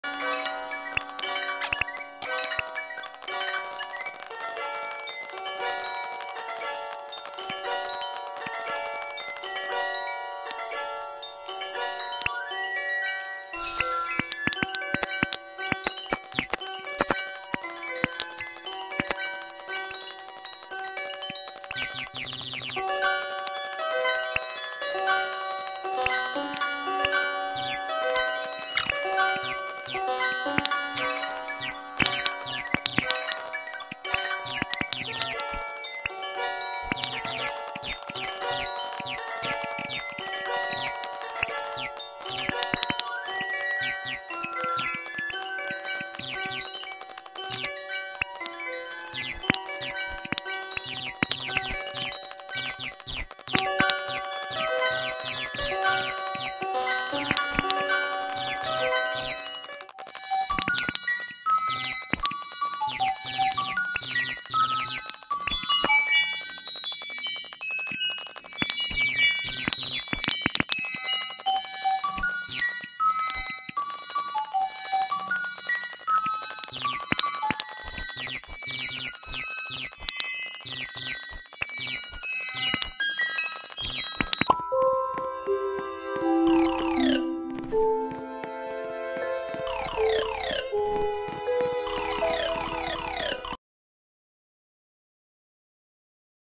Germanyball tries to work at a pizza place but horribly fails
@Hydrus_CB is included in this video